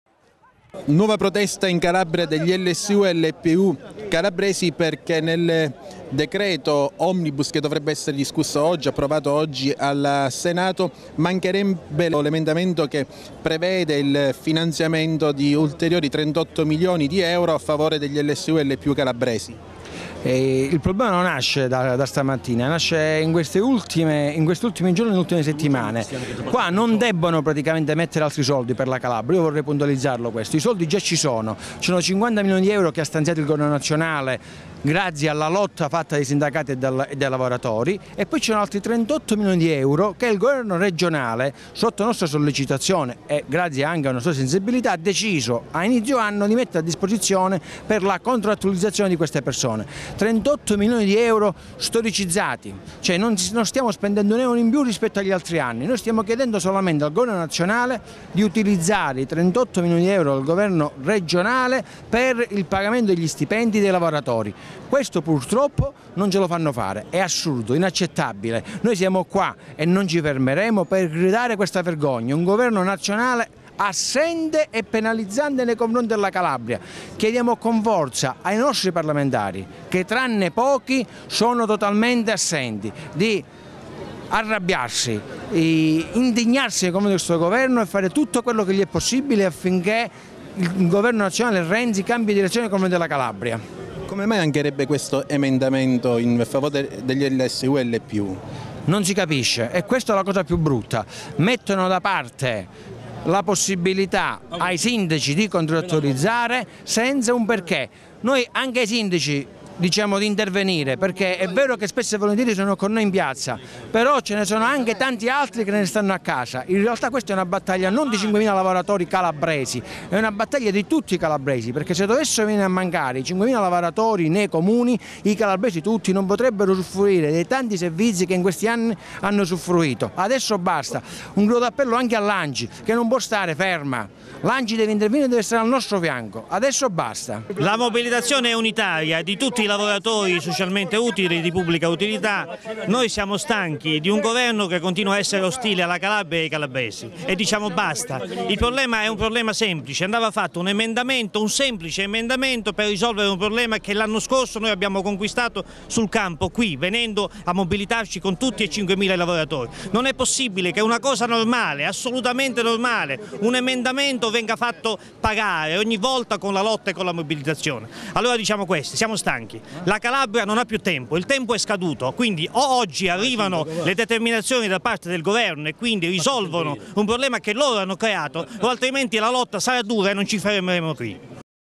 protesta lsu lpu cosenza
intervistati
Gianvincenzo Petrassi segretario uiltemp
Angelo Sposato segretario cgil pollino sibaritide